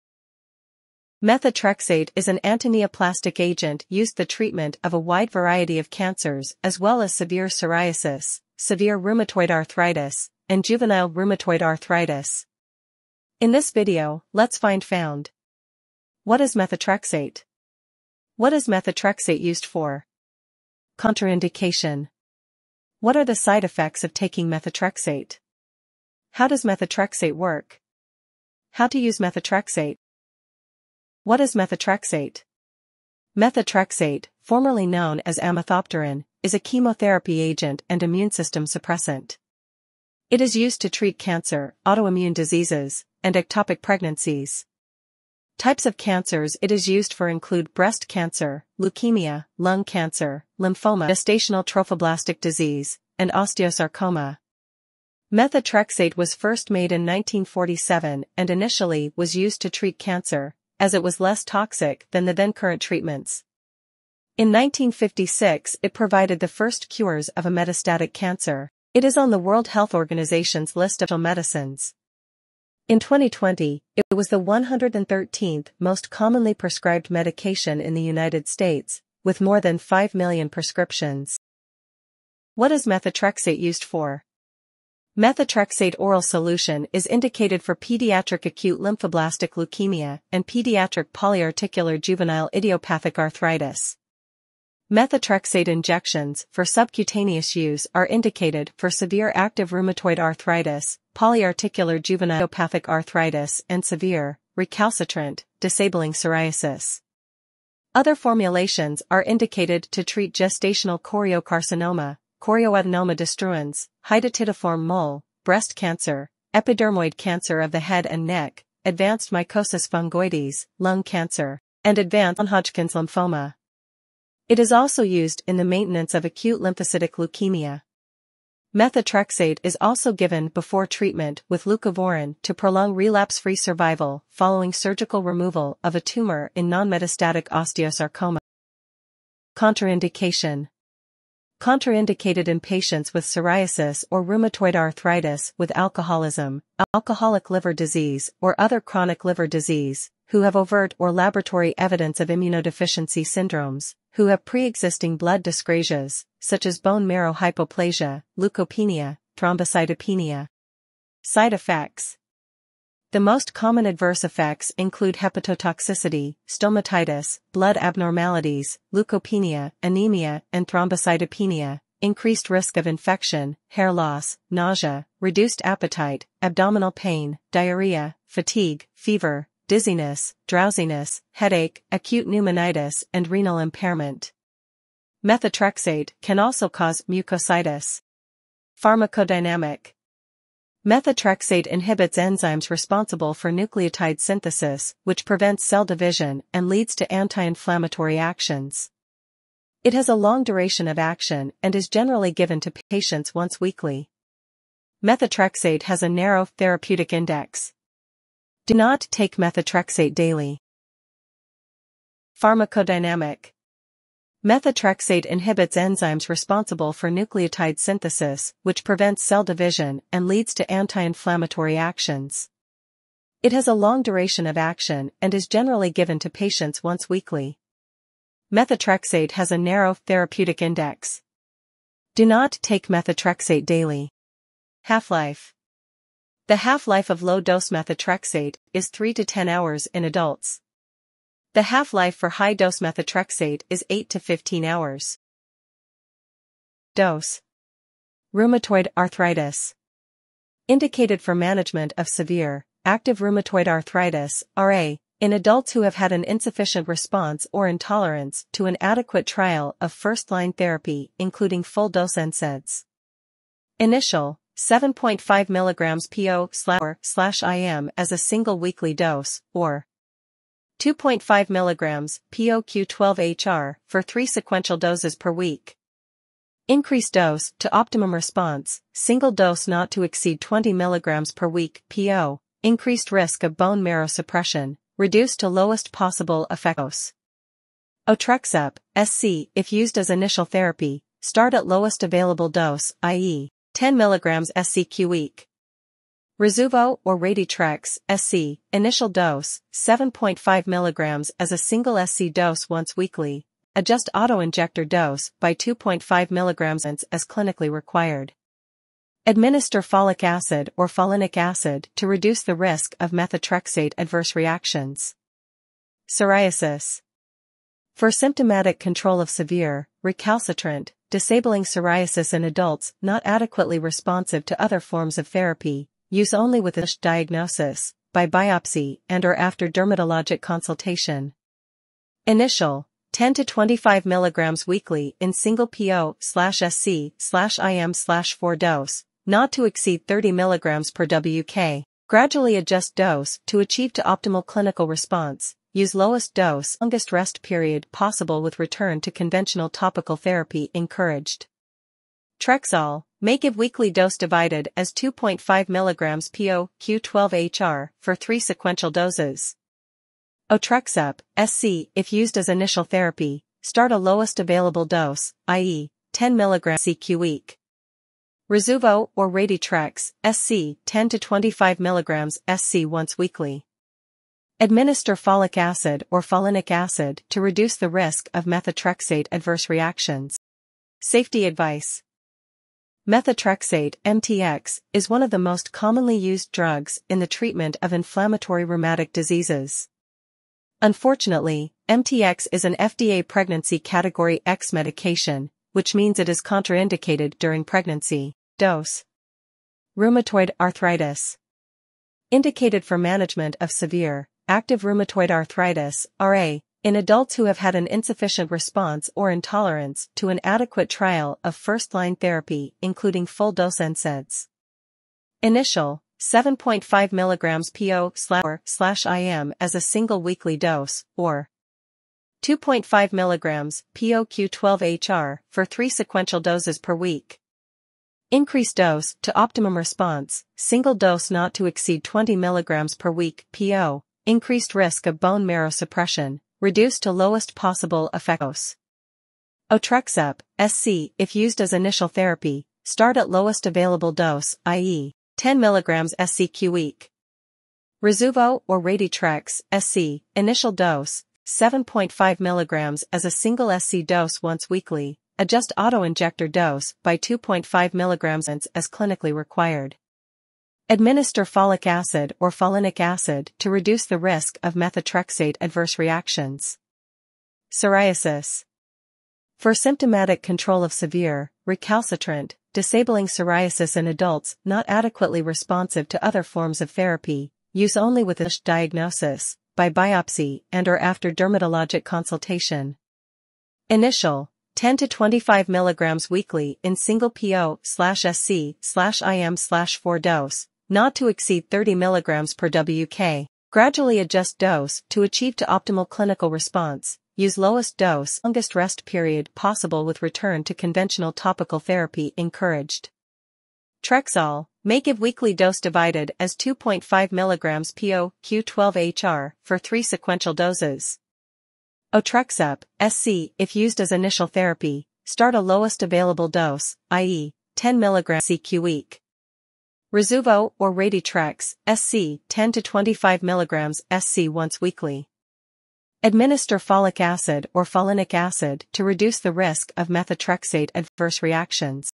methotrexate | Uses, Dosage, Side Effects & Mechanism | Trexall
Methotrexate is an antineoplastic agent used the treatment of a wide variety of cancers as well as severe psoriasis, severe rheumatoid arthritis, and juvenile rheumatoid arthritis.

In this video, let’s find found:
What is methotrexate?
What is methotrexate used for?
Contraindication
What are the side effects of taking methotrexate?
How does methotrexate work?
How to use methotrexate?

📚 Sources / References
•  Medication information (uses, pharmacodynamics, mechanism of action, half-life):